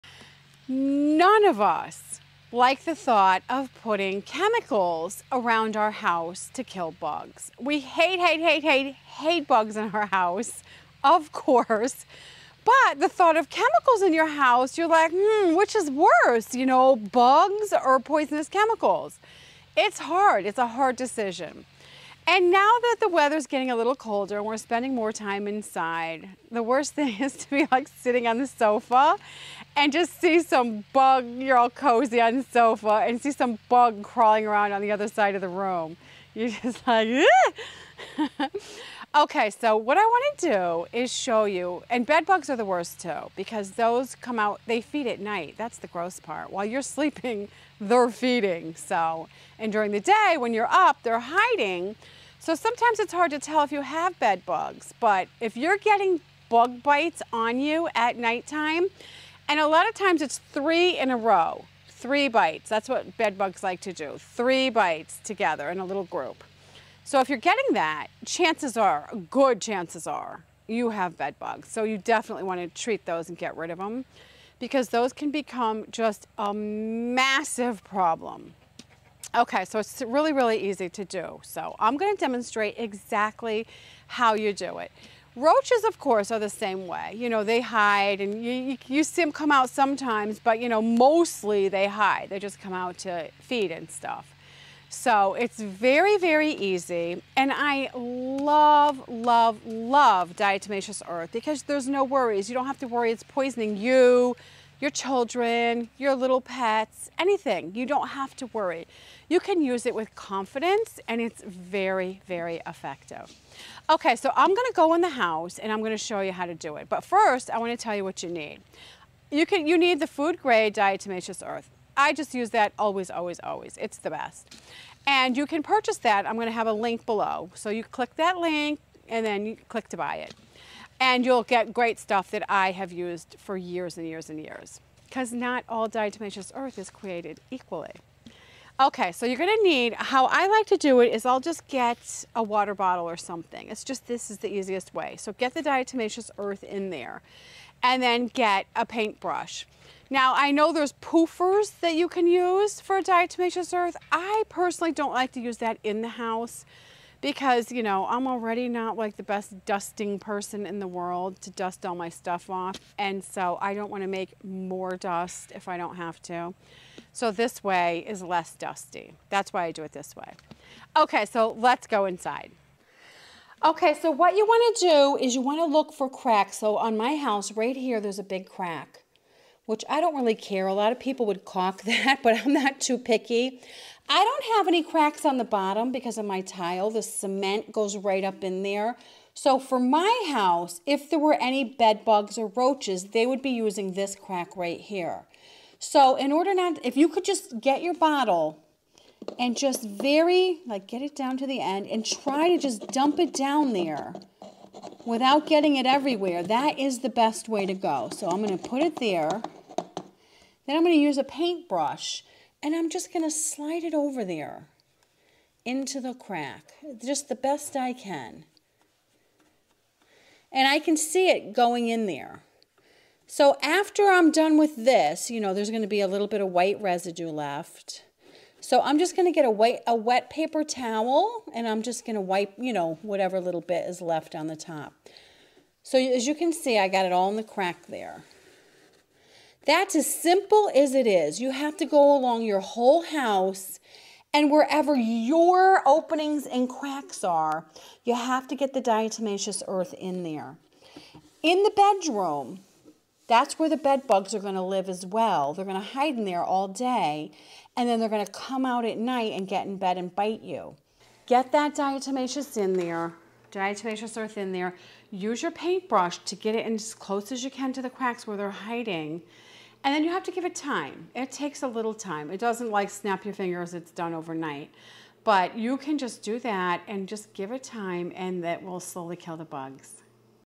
How to Use Diatomaceous Earth for Roaches and Bed Bugs
In this video I explain how to use diatomaceous earth for roaches and bed bugs.